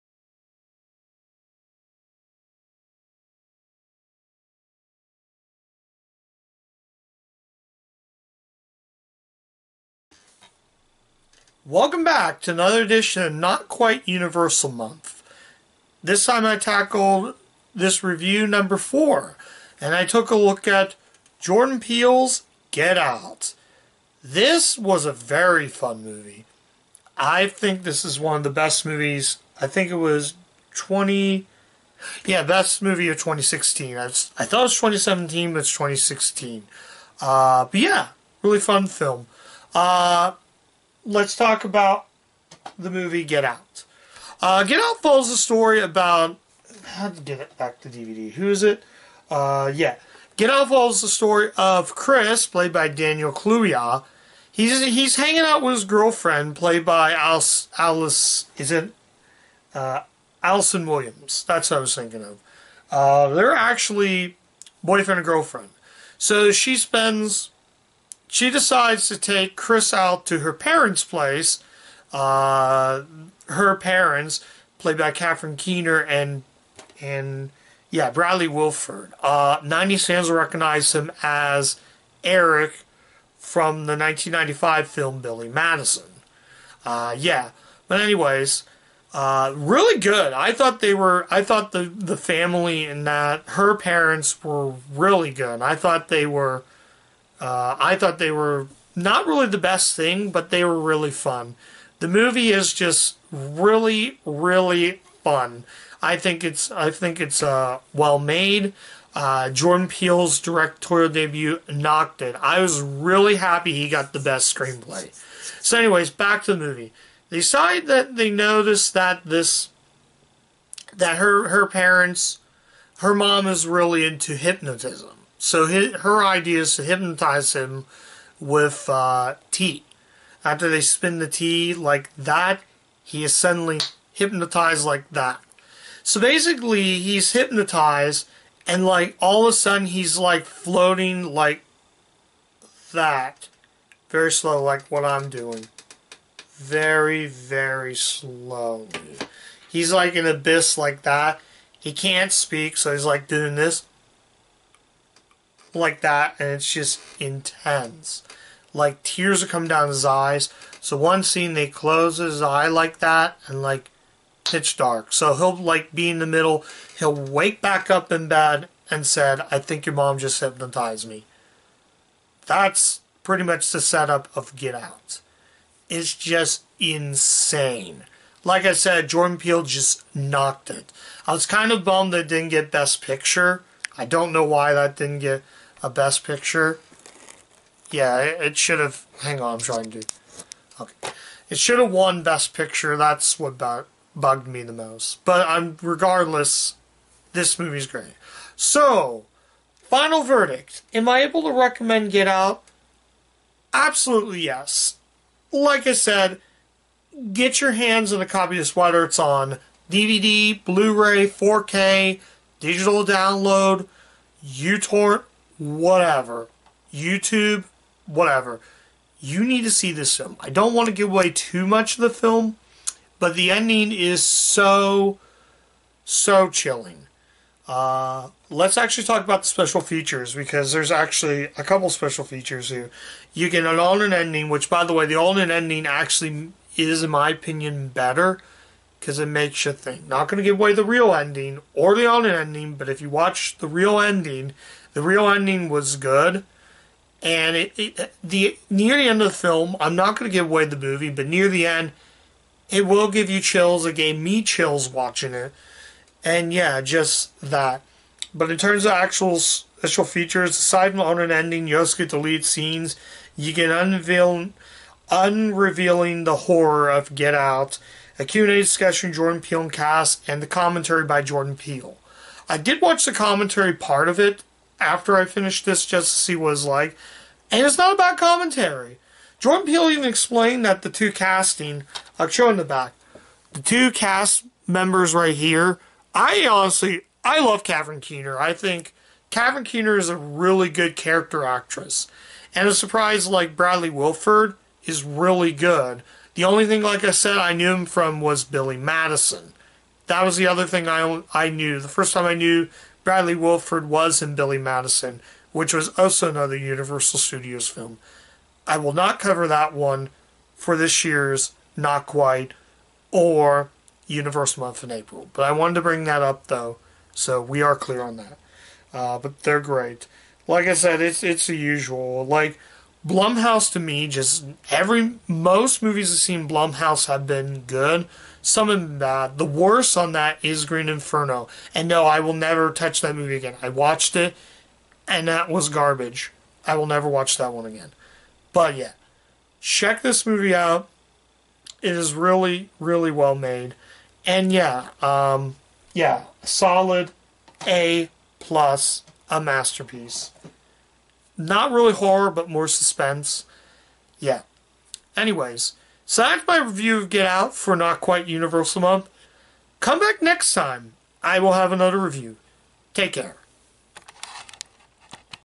Hidden In The Universal Vault | Get Out
Today's Review Is A Thriller From 2016 Made By Jordan Peele Chilling To Watch Will It Give You Chills Find Out In My Review.